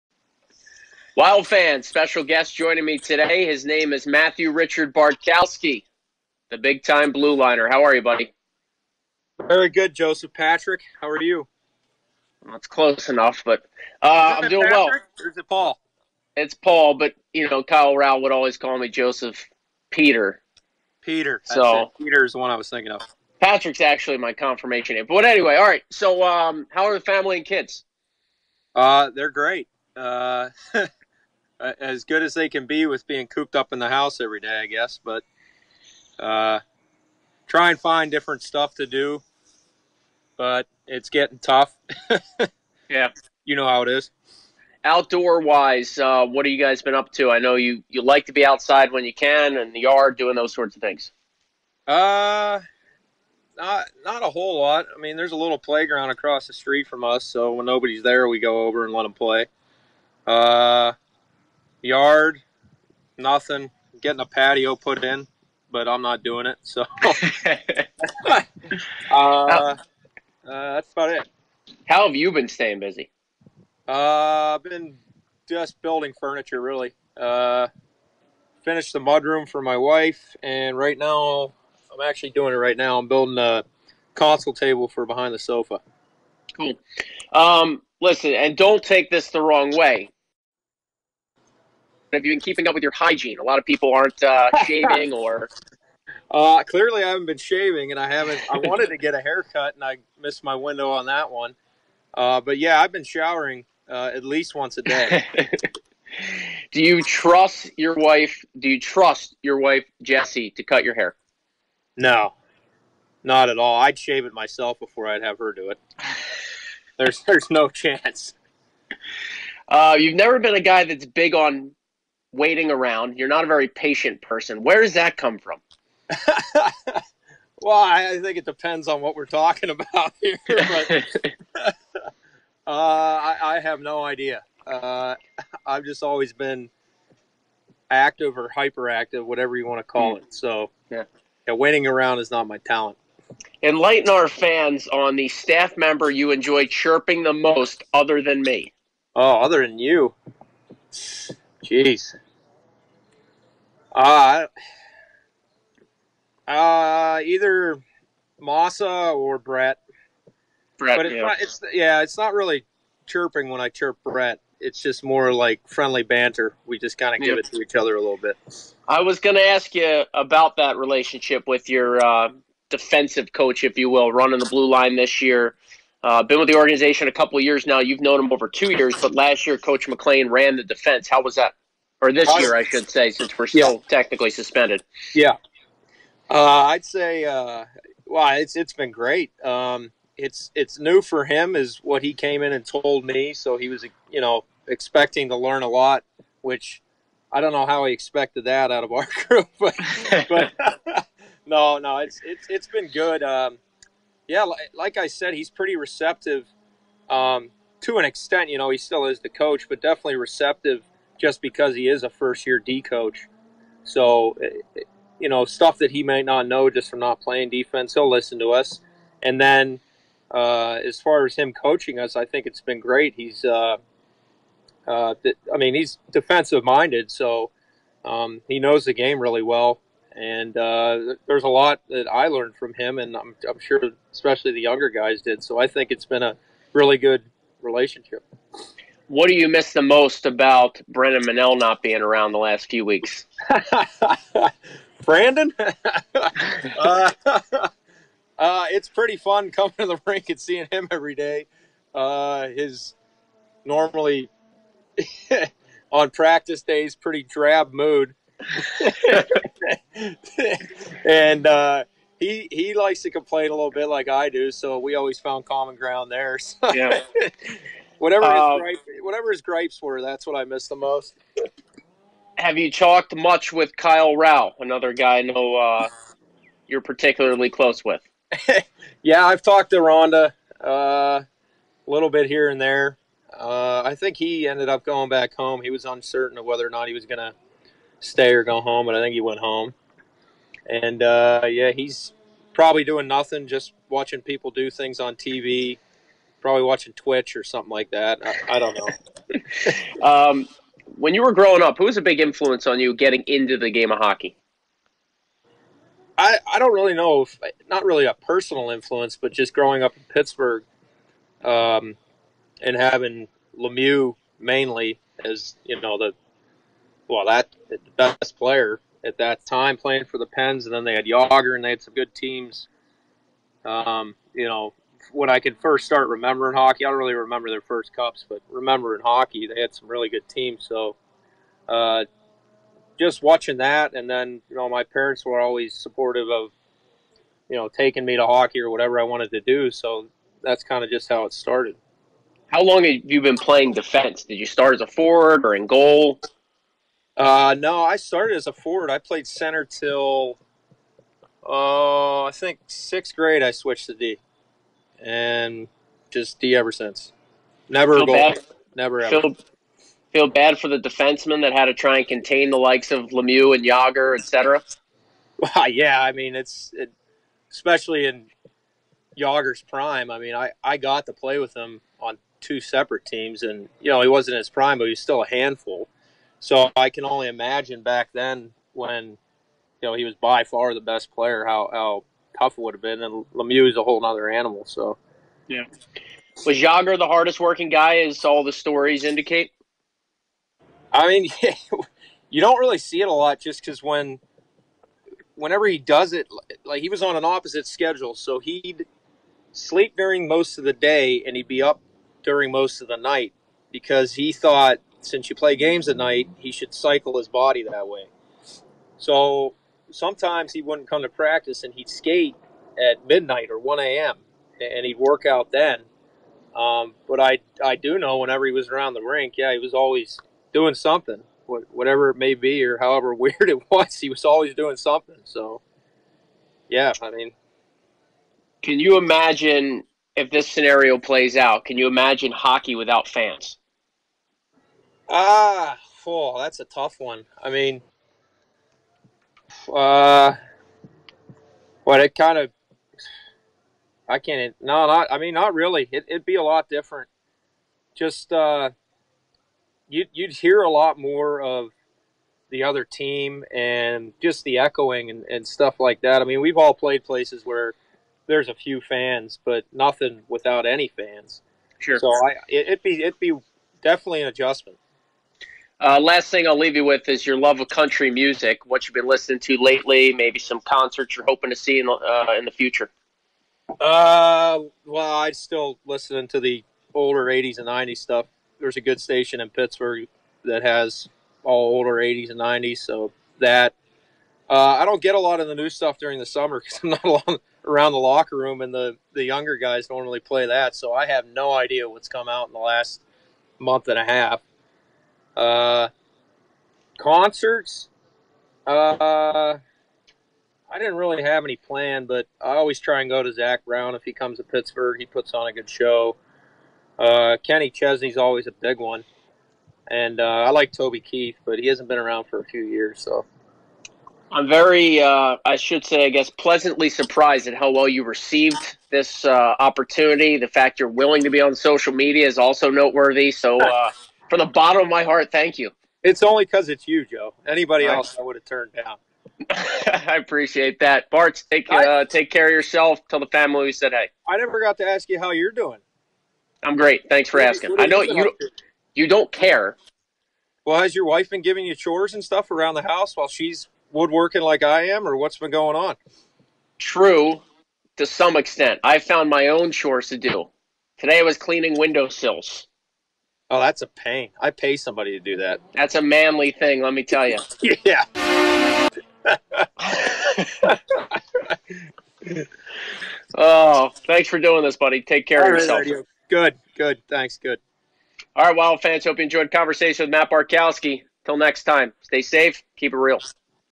Joe O'Donnell Interviews Matt Bartkowski
Joe caught up with Matt recently via FaceTime - check out what he's been up to!